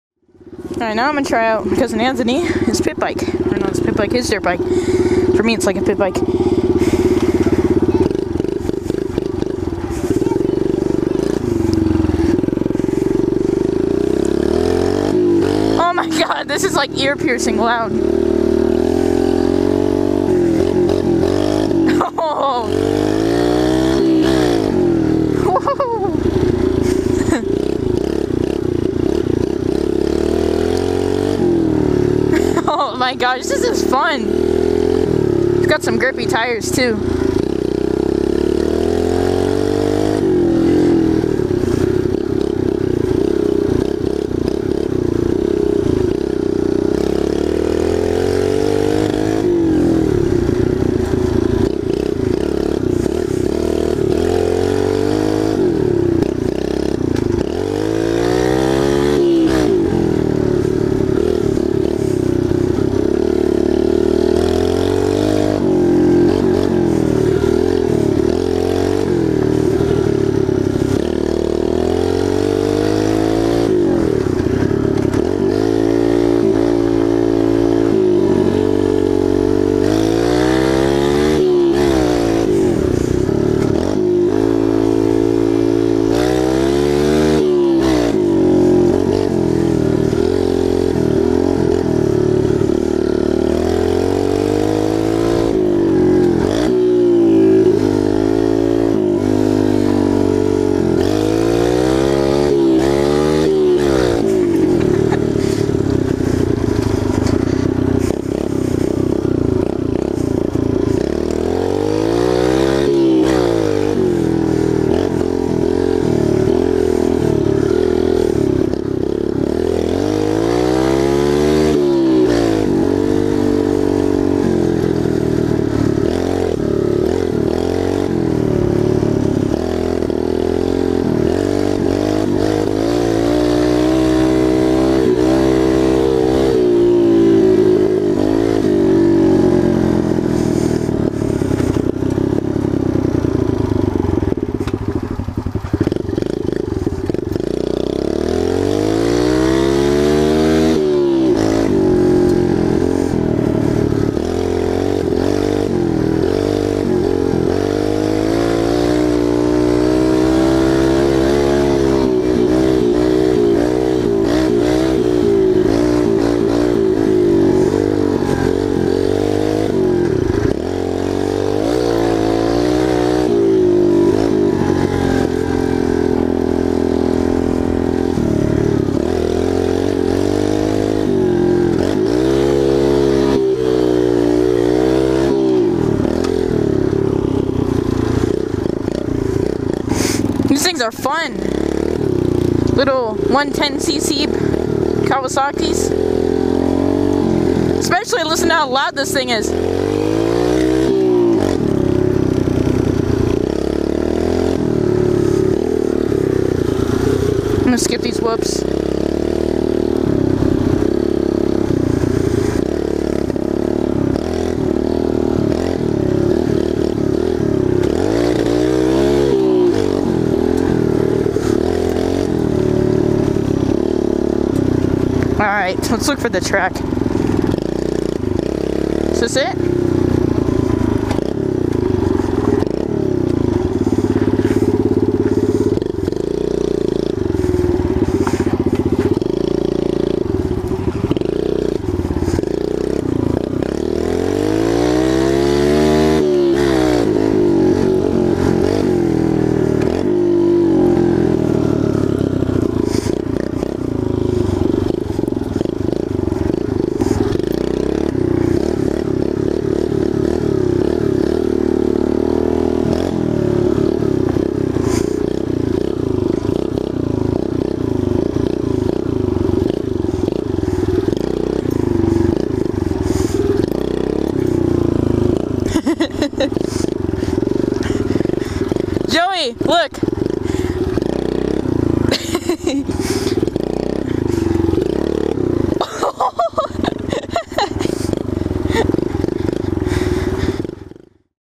Pitbike Fun - Kawasaki KLX 110
Having some fun on my cousin's 2006 KLX 110!
My Dirt Bike: 2005 Yamaha TTR 125E
Mountain Bike: 2015 Norco Sight A7.2